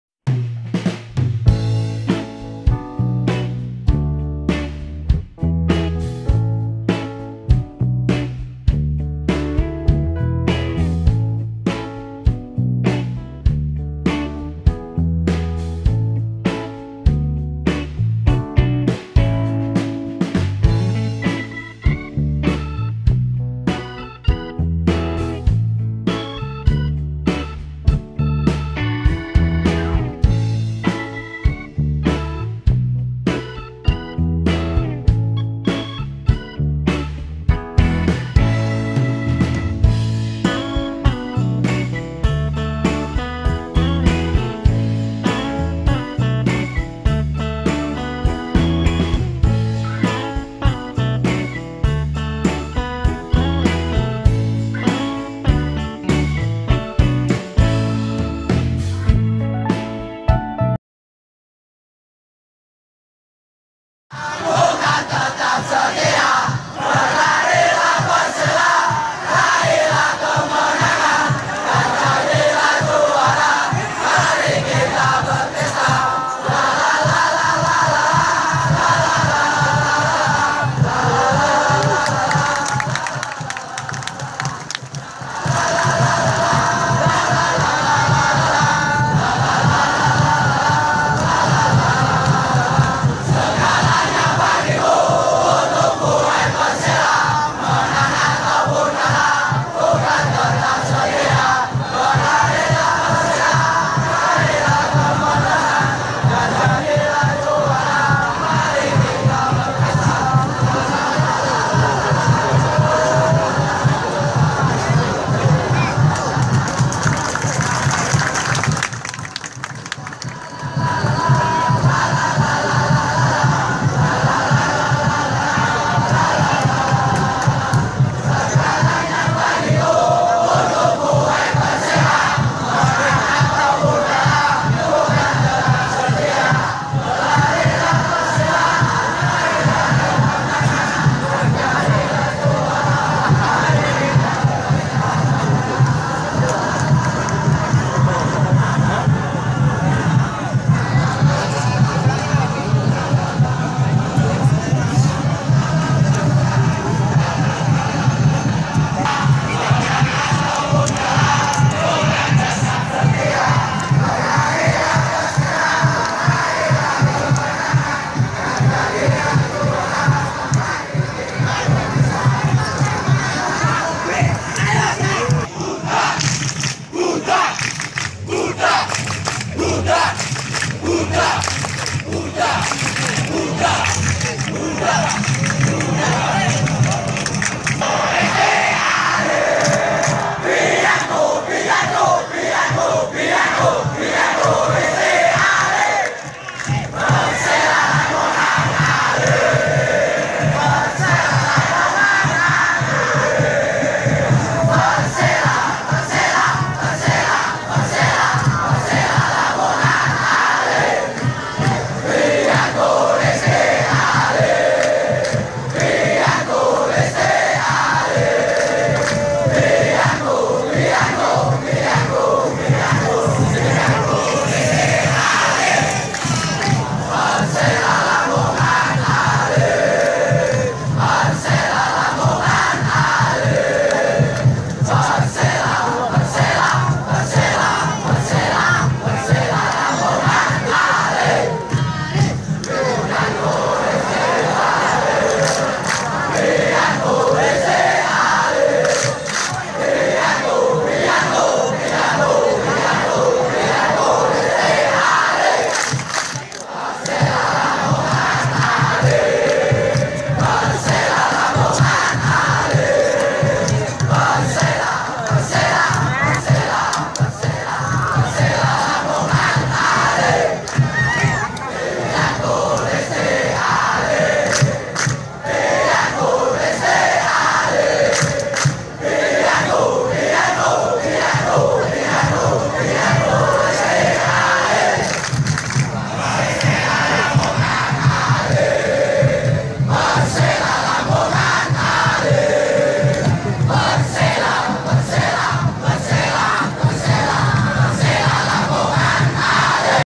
INILAH daftar 19 pemain asing top yang pernah main di PERSELA
INILAH daftar 19 pemain asing top yang pernah main di PERSELA, MARCIO SOZA,GUSTAVO LOPEZ, MARTIN ZADA, FABIANO, MARIO COSTAS, OSCAR ARAVENA, FRANKO HITA, IVAN DISEL CARLOS